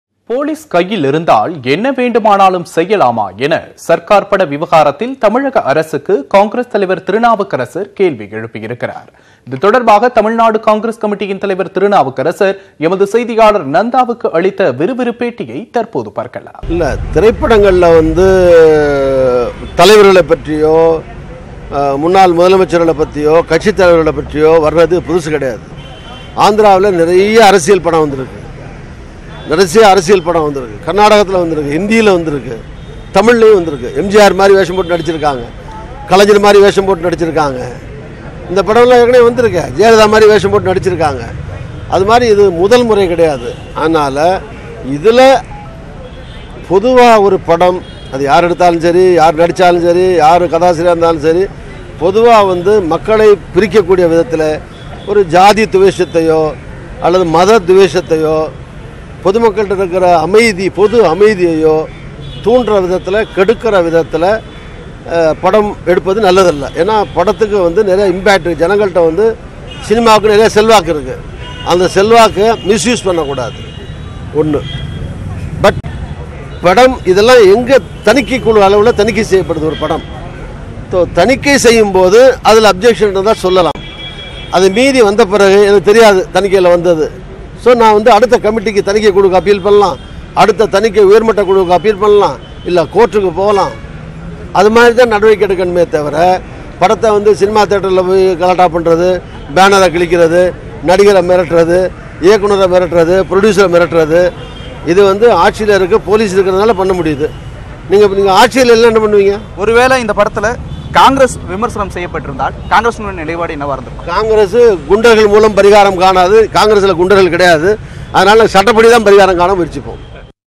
சினிமாவிற்கான செல்வாக்கை தவறாக பயன்படுத்த கூடாது - திருநாவுக்கரசர் | Tn Congress Leader
போலீஸ் கையிலிருந்தால் என்ன வேண்டுமானாலும் செய்யலாமா? என சர்கார் படவிவகாரம் தொடர்பாக தமிழ்நாடு காங்கிரஸ் தலைவர் திருநாவுக்கரசர்  தமிழக அரசுக்கு கேள்வி எழுப்பியுள்ளார்